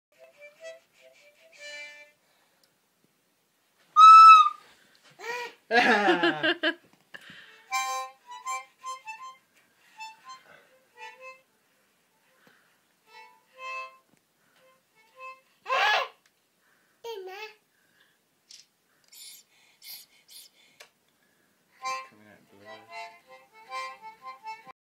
Sophie the music girl!
Sophie at grandad's playing the mouth organ and the recorder.